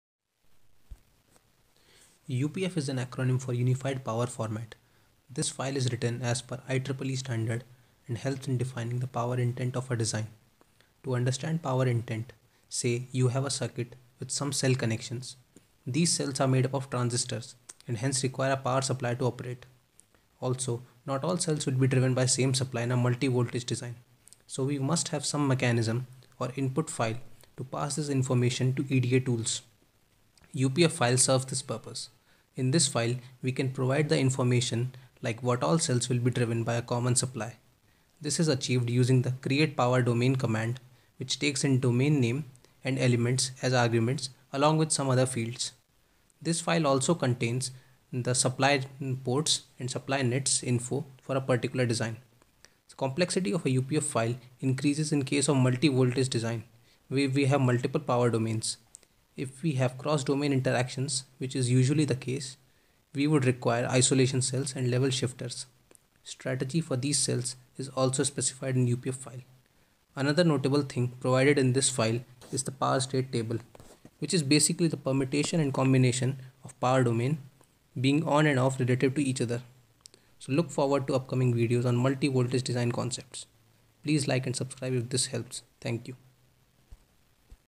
What is UPF?
Brief overview on UPF file and related terminologies.

Short video on multi voltage design.